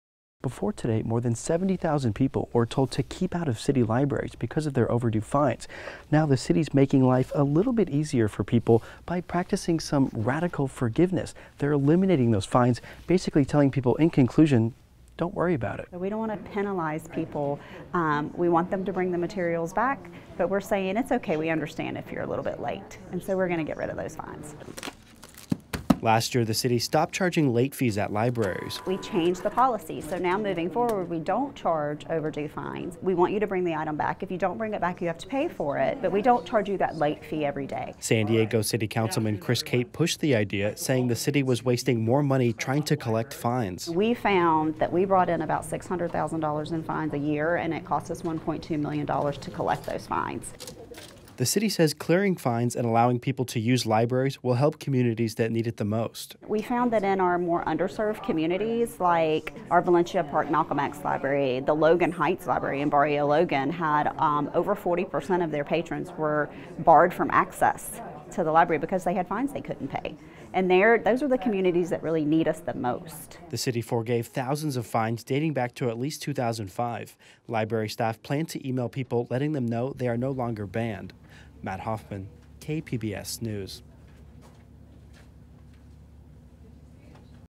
City Of San Diego Libraries Drop Bans Of More Than 70,000 People
Today the city of San Diego waived more than 2 million dollars in library fines. KPBS reporter Matt Hoffman says now it means people who were banned due to late fees are welcome back.